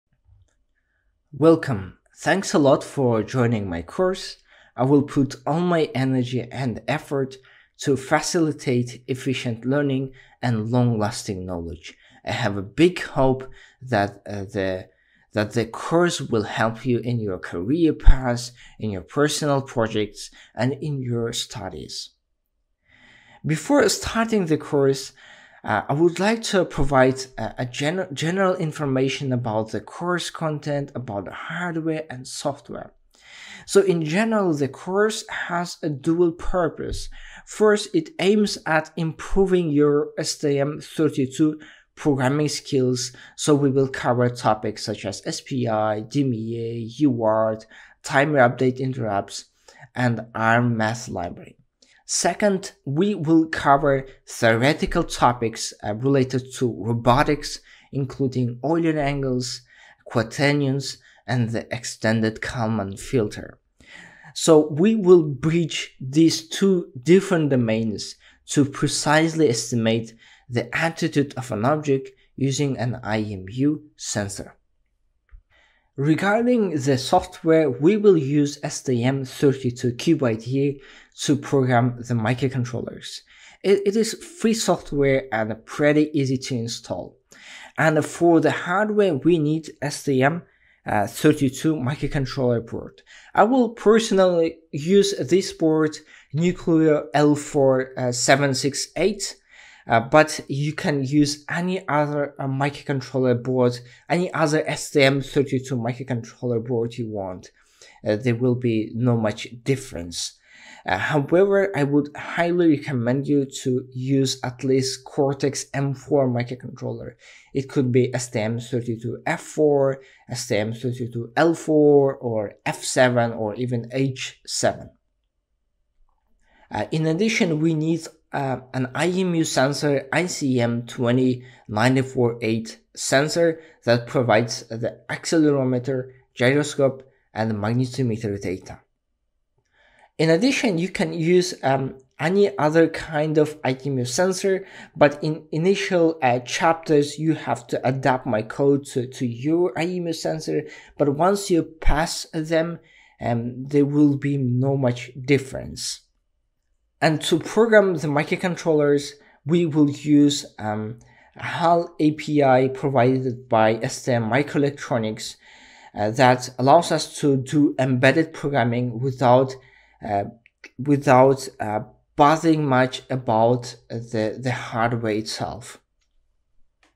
STM32 Attitude Estimation - Welcome to the Course
Master attitude estimation: IMU sensor interfacing, Euler angles, Quaternions, and Kalman Filter. Learn practical STM32 programming for SPI, UART, and Timer interrupts.

+ Mastering basic and advanced topics related to Orientation estimation

+ Learning to work with datasheets and reference manuals of sensors

+ Applying math equations in real-world applications

+ Working with SPI, and UART in polling and DMA modes

+ Streaming IMU sensor readings in real-time

+ Using an IMU sensor (gyroscope, accelerometer, and magnetometer) to compute Euler angles

+ Learning the math behind Quaternions

+ Learning the Extended Kalman Filter

+ Using the Extended Kalman Filter for Orientation Estimation